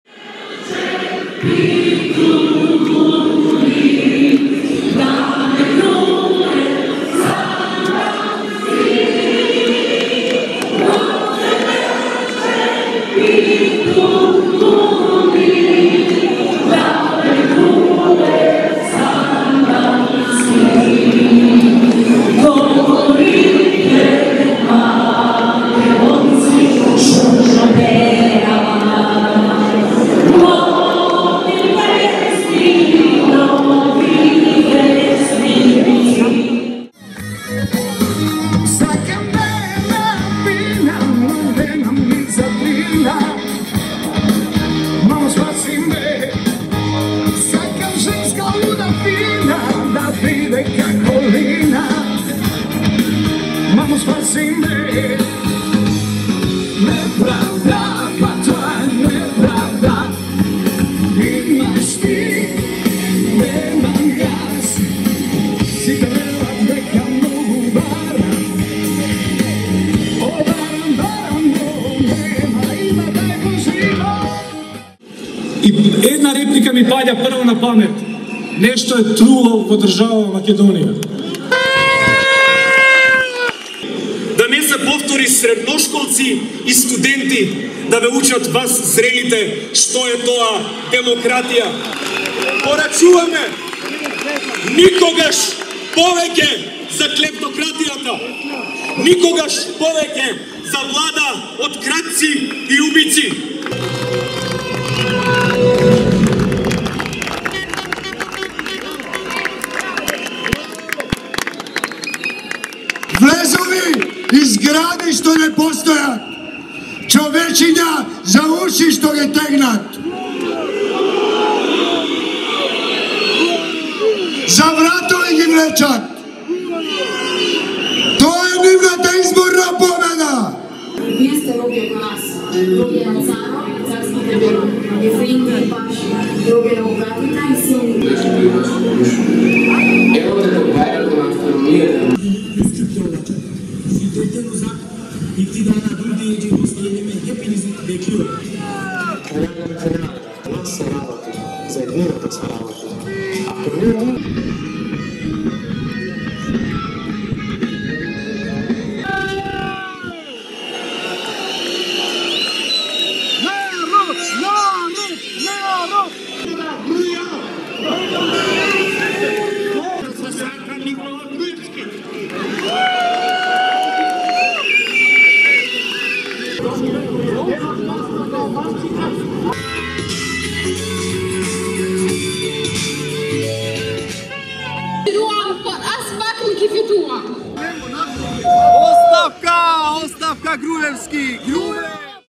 Масовен анти владин протест во Скопје
Луѓето носат транспаренти „Збогум Никола“ и веат македонски, албански, српски, турски, ромски знамиња. Организатори на протестот се граѓански здруженија, невладини организации и политички партии, меѓу кои и најголемата опозициска партија, Социјал-демократски сојуз.